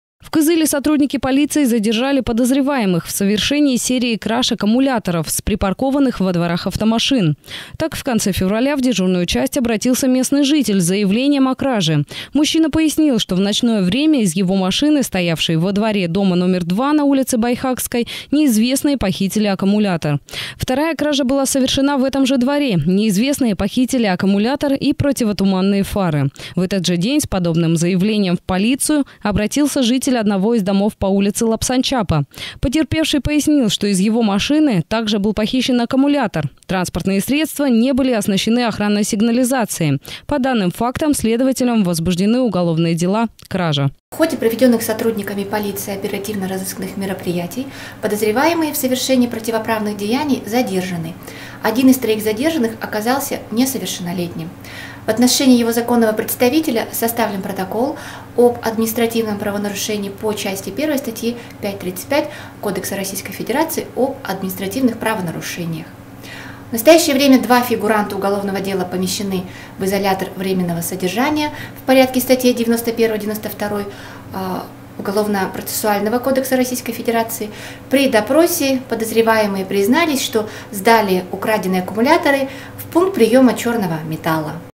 Кража аккумуляторов
В Кызыле сотрудники полиции задержали подозреваемых в совершении серии краж аккумуляторов с припаркованных во дворах автомашин.